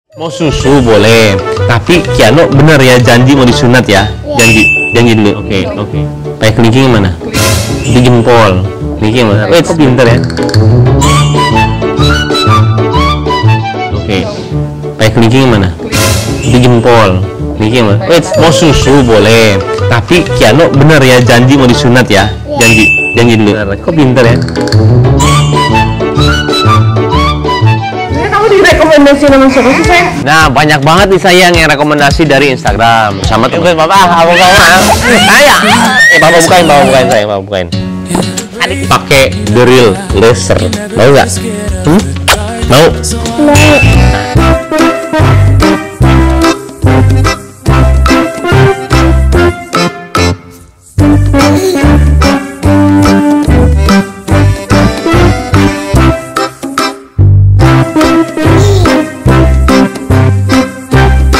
NGAKAK BANGET KIANO JANJI KELINGKING JADI JANJI JEMPOL KARENA BELUM PAHAM HAHAHA 😍
Kiano lucu banget deh bosque.. sukanya buat ketawa karena tingkah lucunya... NGAKAK BANGET KIANO JANJI KELINGKING JADI JANJI JEMPOL KARENA BELUM PAHAM HAHAHA  😍